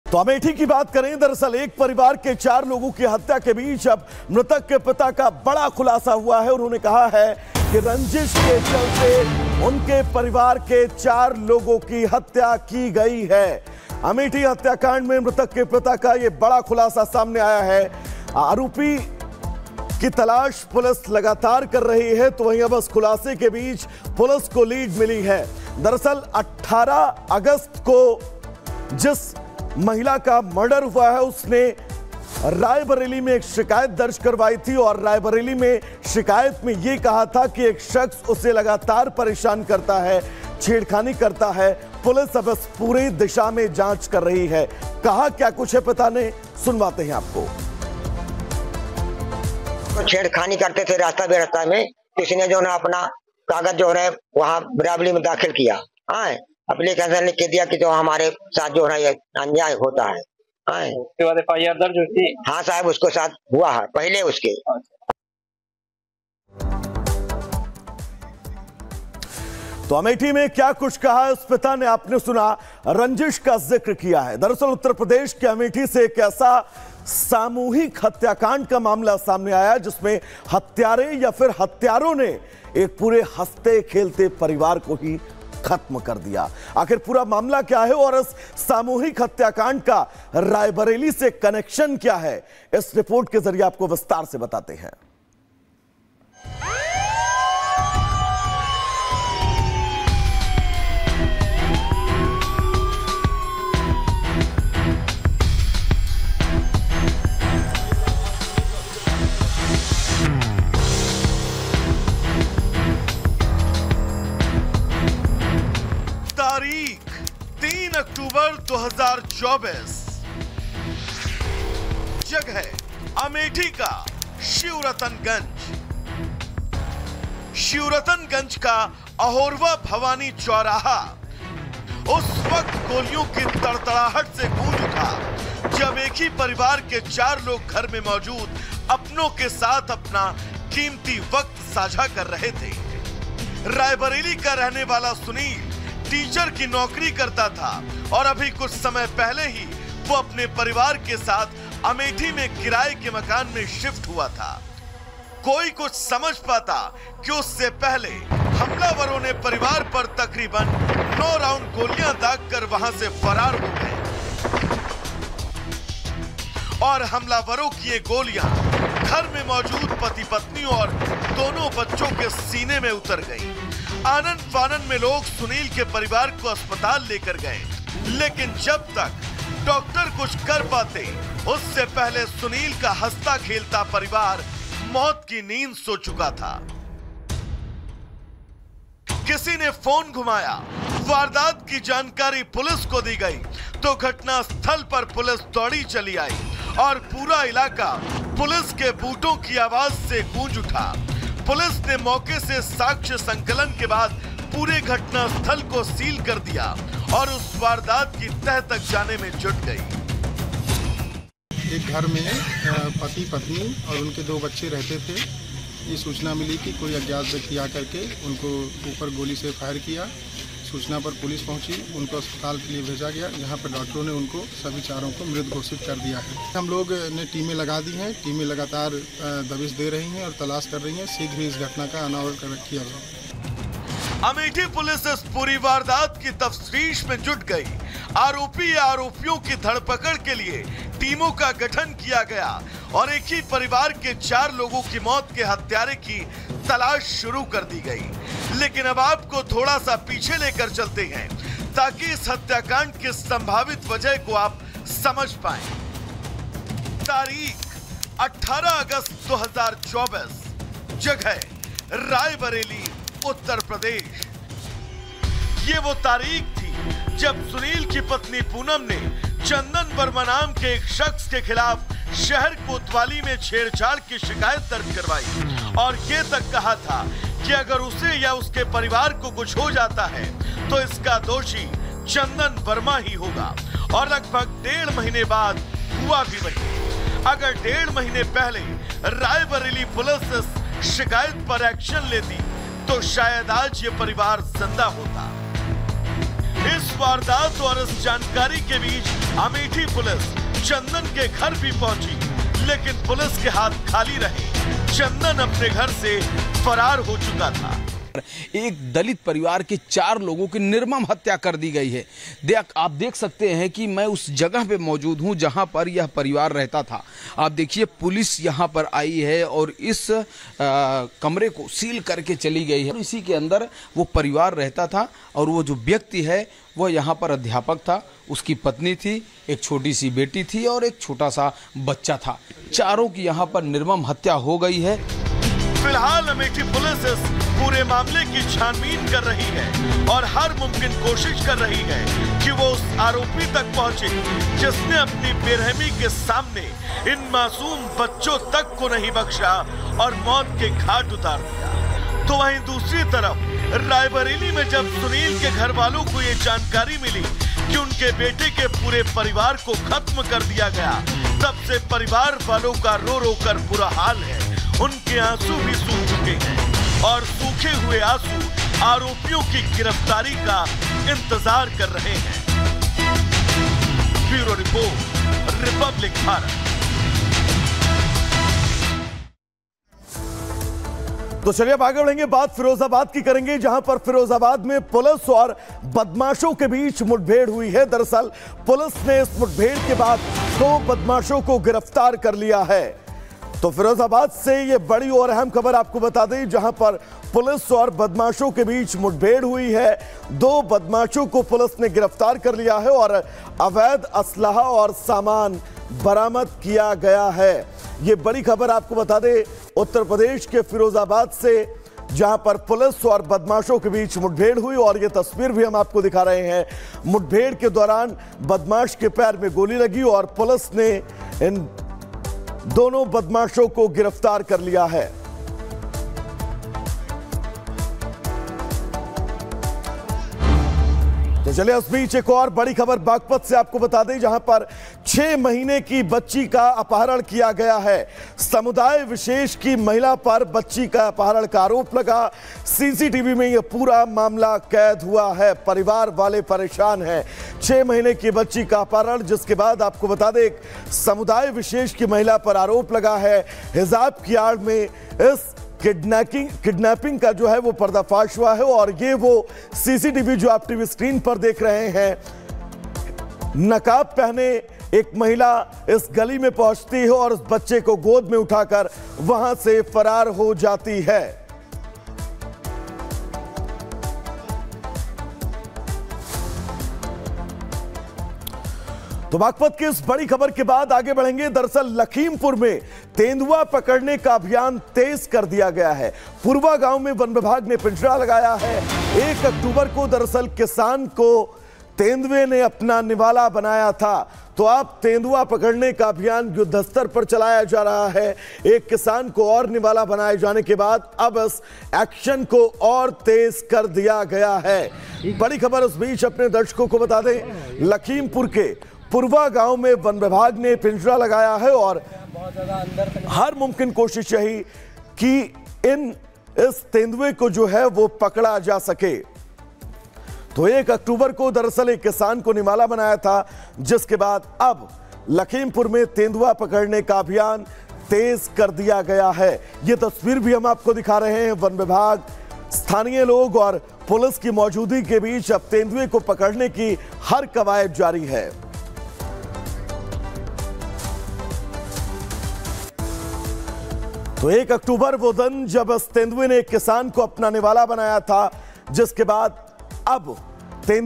Amethi Murder News: अमेठी में एक परिवार के 4 लोगों की हत्या, सख्त कार्रवाई के आदेश | R Bharat
यूपी के अमेठी में एक ही परिवार के 4 लोगों का मर्डर. सीएम योगी ने लिया मामले का संज्ञान...मृतक के पिता का दावा... रंजिश के चलते हुई हत्या... बहू ने 18 अगस्त को दर्ज करवाया था मुकदमा